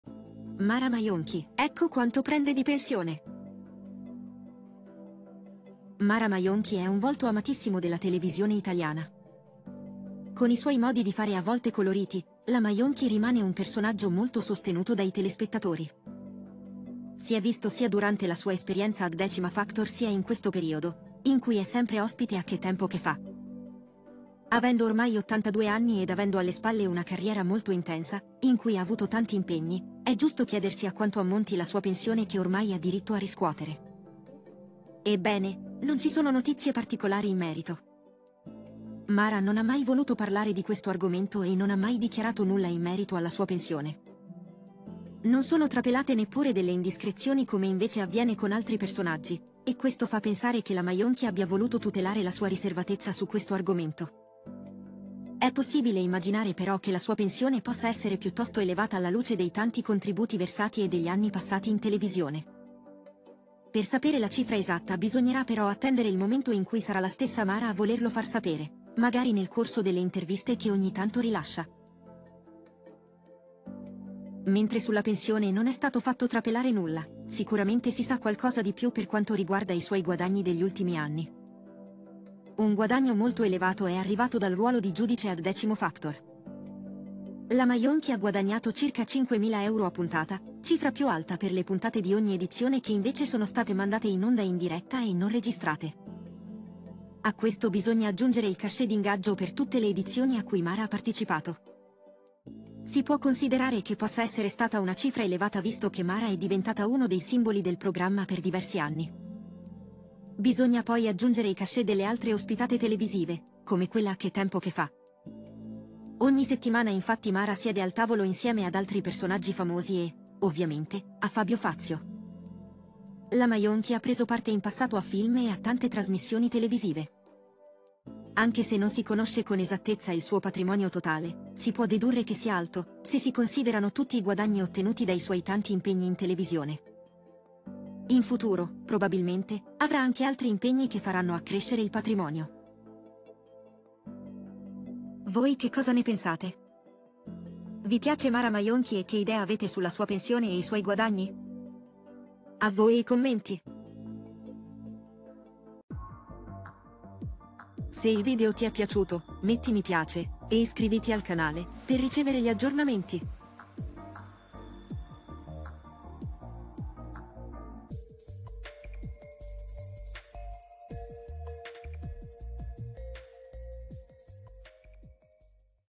Mara Maionchi: Ecco Quanto Prende Di Pensione!
Mara Maionchi: molto in vista tra i personaggi televisivi italiani, ora Mara ha 82 anni e può percepire quindi una pensione. Ecco a quanto ammonta!